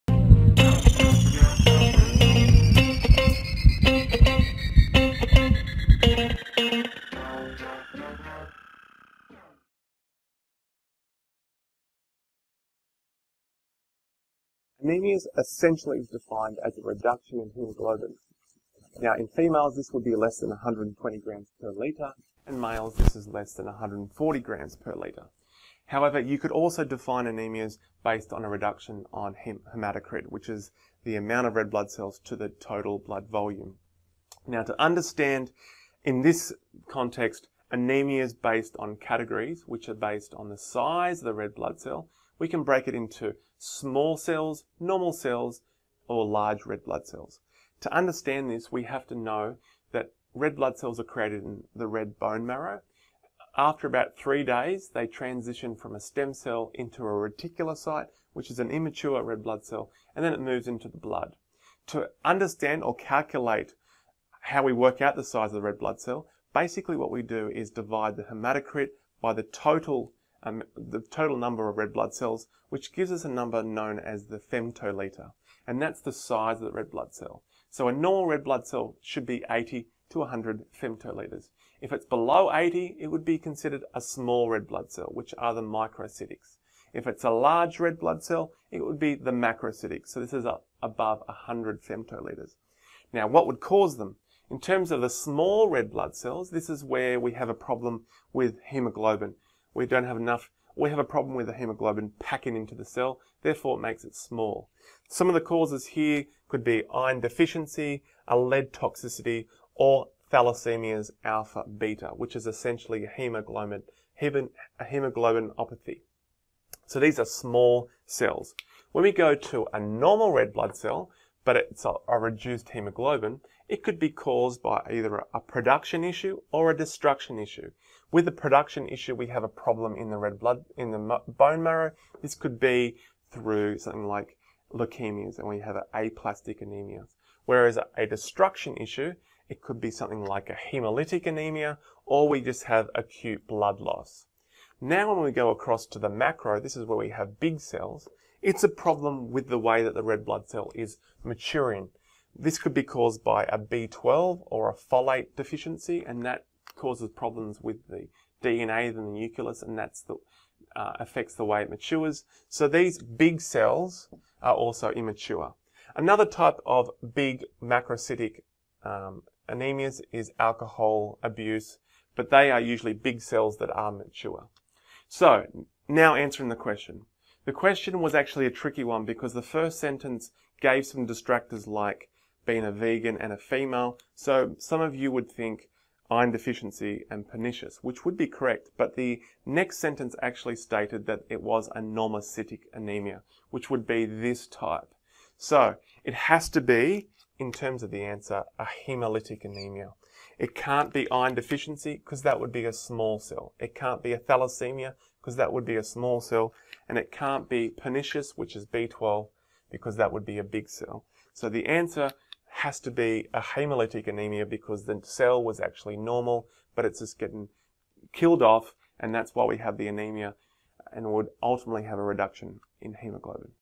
Exam Question | Anaemia
A 24-year-old female, who is vegan, presents to her GP with extreme fatigue and shortness of breath on minimal exertion. A FBC diagnoses her with normocytic anaemia. Based on this diagnosis, what type of anaemia does she have?

a) Iron-deficiency anaemia

b) Haemolytic anaemia

c) Pernicious anaemia

d) Thalassiamia

e) A & C